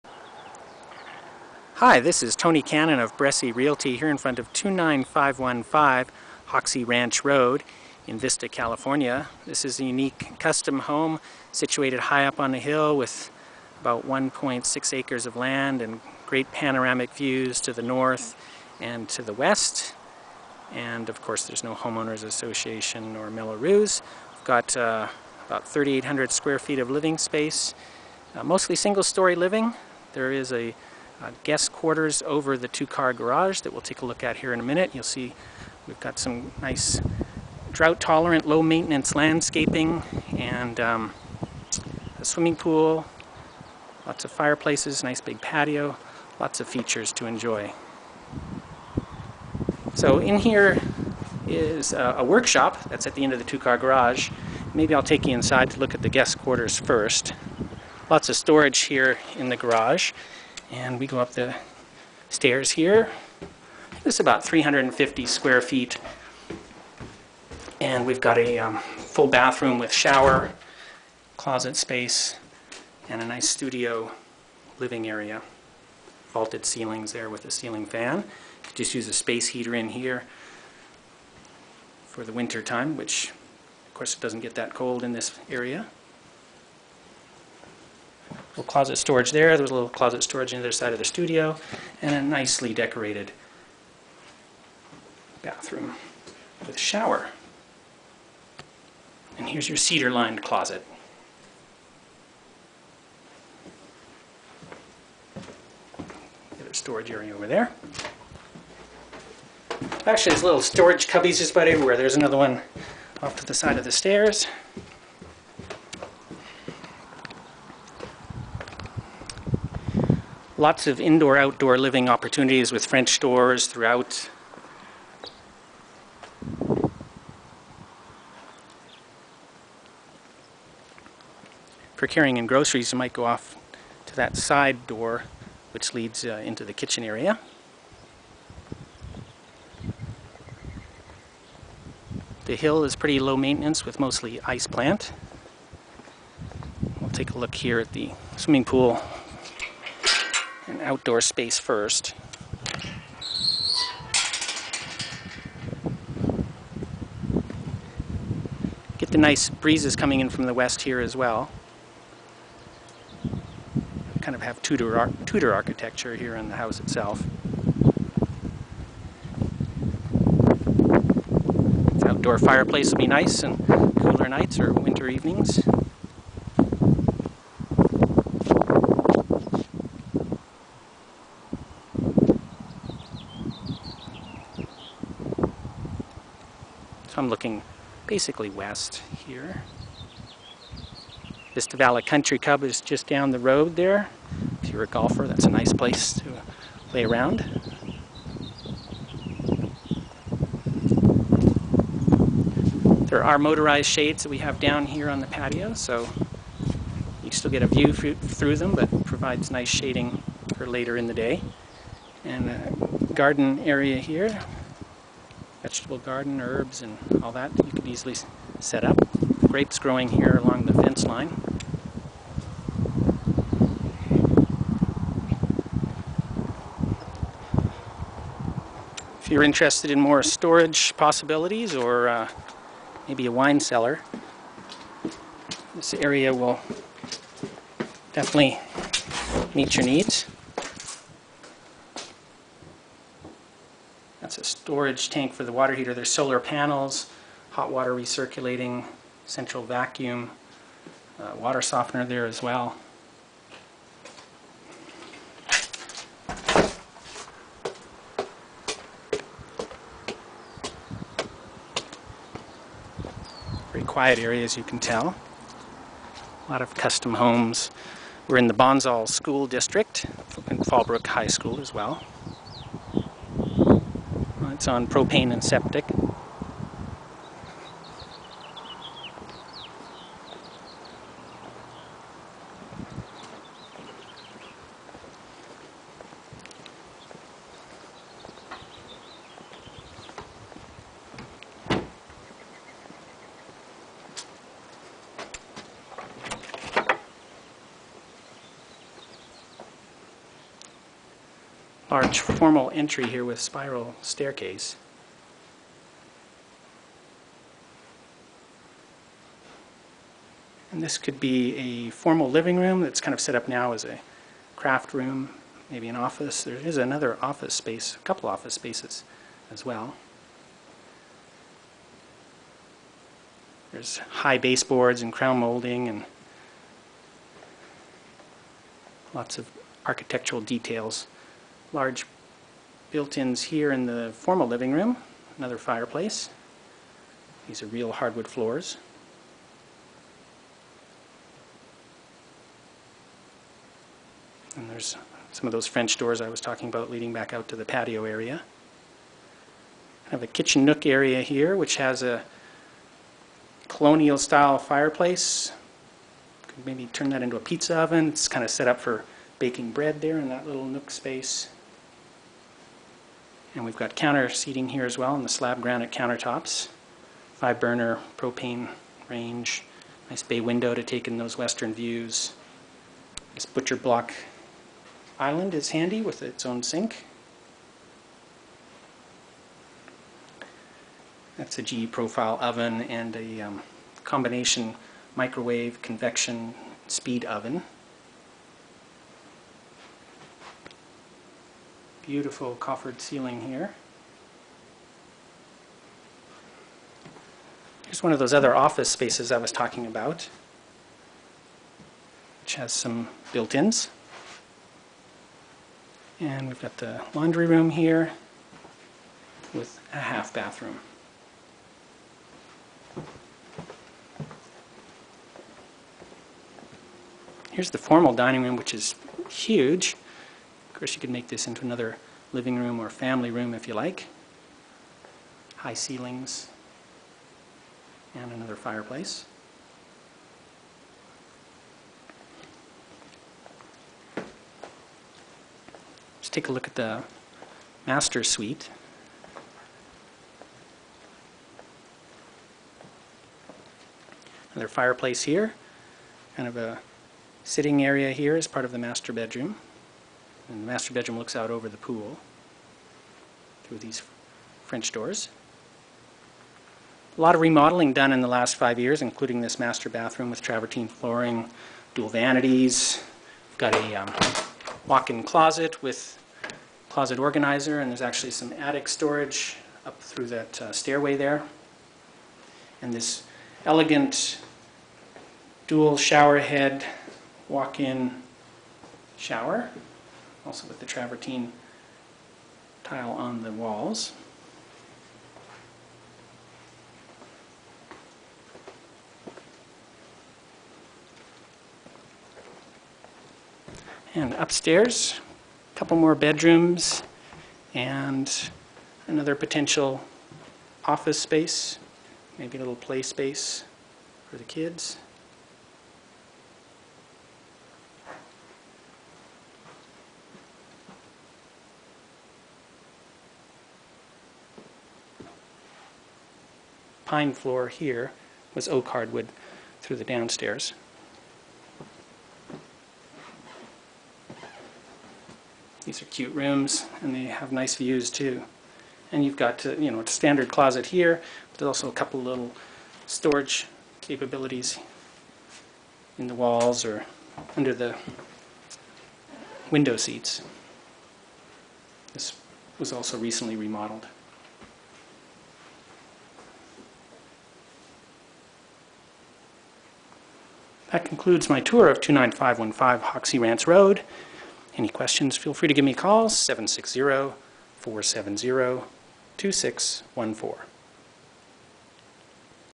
French Manor View Home With Pool in Vista, CA
29515 Hoxie Ranch Rd in Vista, CA is a French provincial manor style home on almost 1.5 acres with 3 bedrooms in the main house and a guest suite over the 2 car garage. Awesome western views are enjoyed from many rooms inside the house as well as on the outdoor patio and around the swimming pool. Nearby Vista Valley Country Club